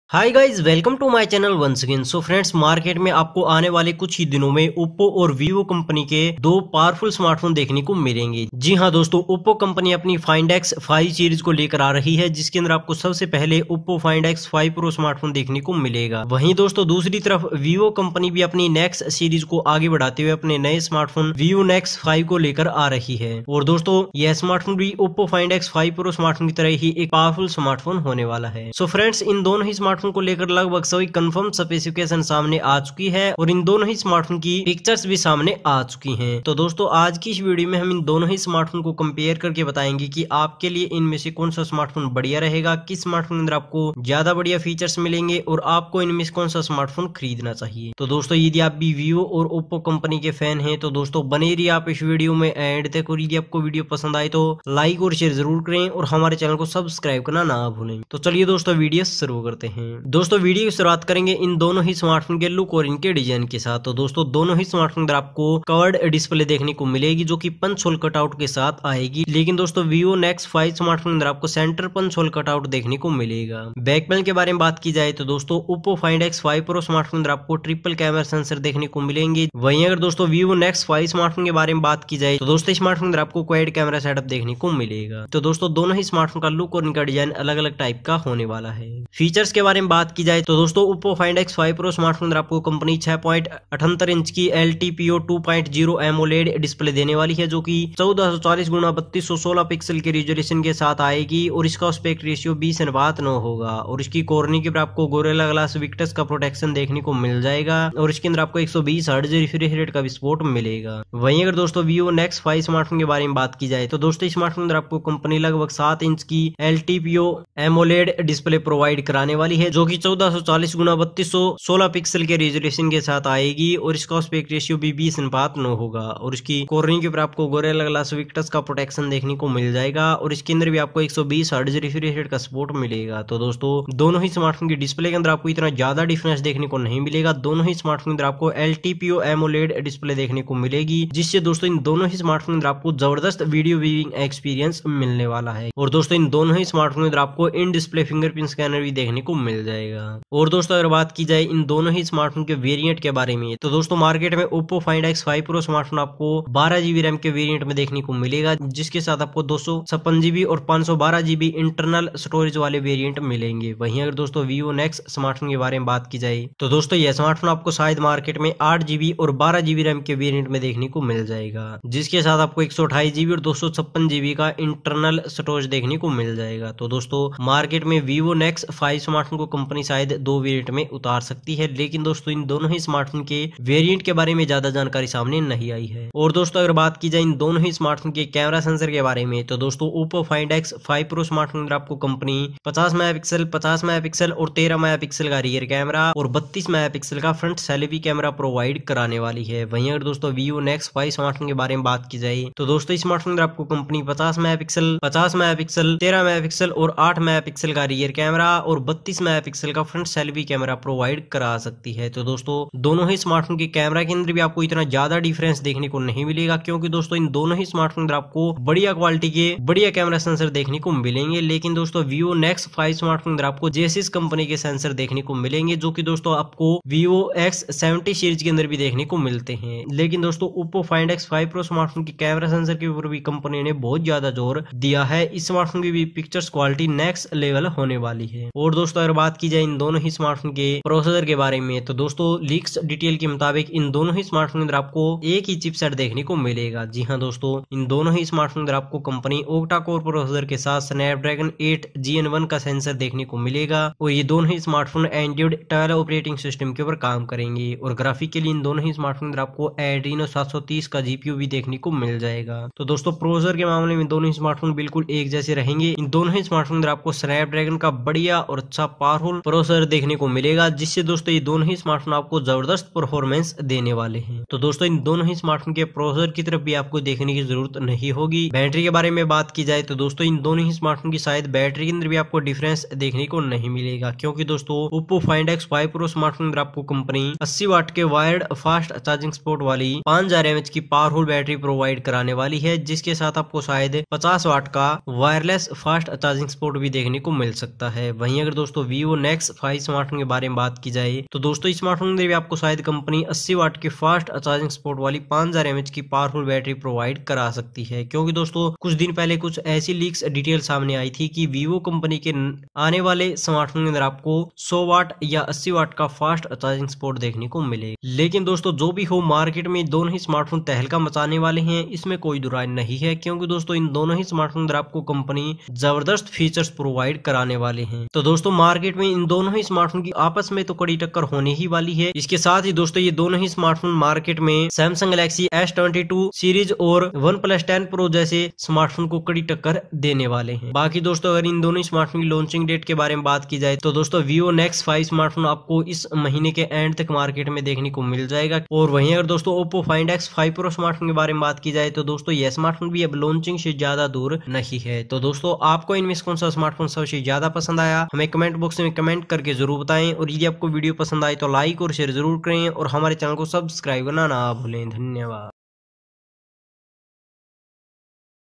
Vivo Nex 5 Vs OPPO Find X5 Pro || Full Comparison ⚡ Which one is Best
In this video, we are Compare Vivo Nex 5 Vs OPPO Find X5 Pro New smartphones Vivo Nex 5 Vs OPPO Find X5 Pro  You will find complete details of We will talk about Vivo Nex 5 Vs OPPO Find X5 Pro , Vivo Nex 5 Vs OPPO Find X5 Pro Performance, Vivo Nex 5 Vs OPPO Find X5 Pro Battery and Vivo Nex 5 Vs OPPO Find X5 Pro Display in the video. We will also talk about. you can also check out Vivo & OPPO .

Display
Size
6.78 inch LTPO 120Hz
Resolution
1440 x 3216 Pixel
Aspect Ratio
20:9
Corning -
Gorilla Glass Victus

Camera
Triple Rear Camera
50 + 50 + 13  MP
Front Camera
32 MP

RAM
12 GB
ROM
256/512 GB
SD Card
No

Processor
CPU
2.86 GHz Octa -core
Chipset
Snapdragon 8 Gen1
OS
Android 12
GPU
Adreno 730

Battery
Charger
80w Wired Charging
50W wireless
Battery Size
Li-Po 5000mAh

Vivo Nex 5

Display
Size
7.0 inch LTPO 120Hz
Resolution
1440 x 3216 Pixel
Aspect Ratio
20:9
Corning -
Gorilla Glass Victus

Camera
Quad Rear Camera
50+ 50 + 13 + 8  MP
Front Camera
32 MP

RAM
8/12 GB
ROM
128/256 GB
SD Card
No

Processor
CPU
2.86 GHz Octa -core
Chipset
Snapdragon 8 Gen1
OS
Android 12
GPU
Adreno 730

Battery
Charger
33w Fast Charging
Battery Size
Li-Po 5000mAh